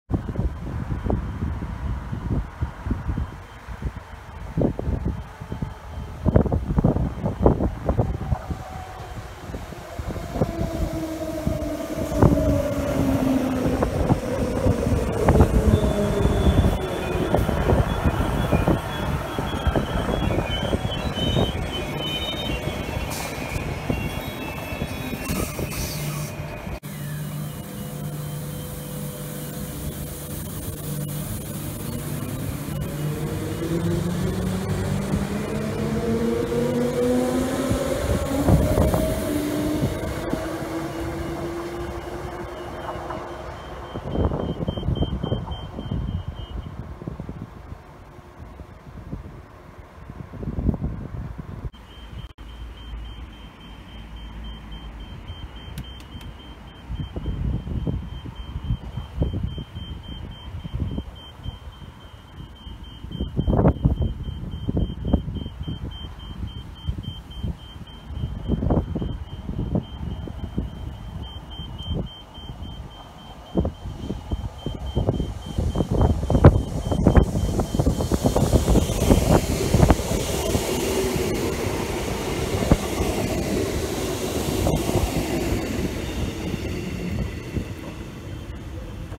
Transperth A-series trains at Loch Street Station
Transperth A-series trains at Loch Street Station on the Fremantle Line in Perth, Western Australia.

Unusually, the train in the second clip displays "Concert" on its destination display.